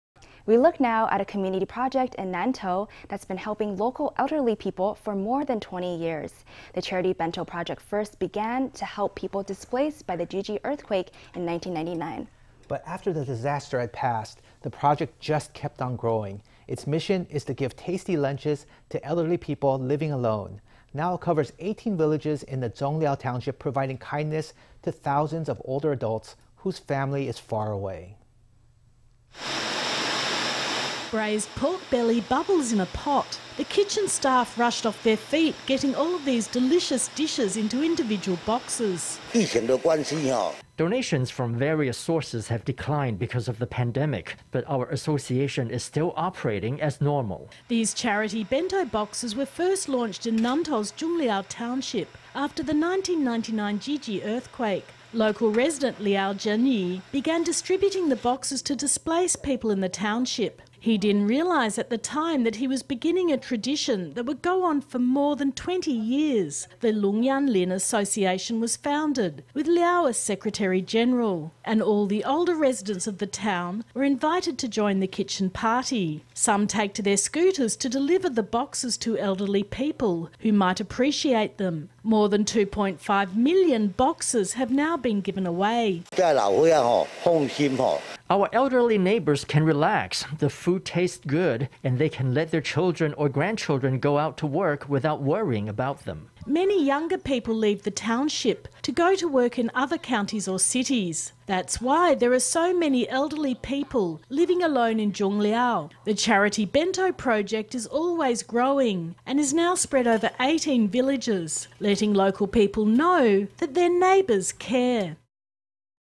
Nantou charity project provides lunchboxes for older adults who live alone
We look now at a community project in Nantou that’s been helping local elderly people for more than 20 years. The charity bento project first began to help people displaced by the Jiji Earthquake in 1999. But after that disaster was past, the project just kept on growing. Its mission is to give tasty lunches to elderly people living alone. Now it covers 18 villages in Zhongliao Township, providing kindness to thousands of older adults whose family is far away.

Braised pork belly bubbles in a pot. The kitchen staff rushed off their feet getting all of these delicious dishes into individual boxes.

Liao Chen-yi
Long-Yan-Lin Welfare Association

These charity bento boxes were first launched in Nantou’s Zhongliao Township after the 1999 Jiji Earthquake. Local resident Liao Chen-yi began distributing the boxes to displaced people in the township. He didn’t realize at the time that he was beginning a tradition that would go on for more than 20 years. The Long-Yan-Lin Welfare Association was founded with Liao as secretary-general, and all the older residents of the town were invited to join the kitchen party. Some take to their scooters to deliver the boxes to elderly people who might appreciate them. More than 2.5 million boxes have now been given away.

Liao Chen-yi
Long-Yan-Lin Welfare Association
Our elderly neighbors can relax. The food tastes good, and they can let their children or grandchildren go out to work without worrying about them.

Many younger people leave the township to go work in other counties or cities. That’s why there are so many elderly people living alone in Zhongliao. The charity bento project is always growing and is now spread over 18 villages, letting local people know that their neighbors care.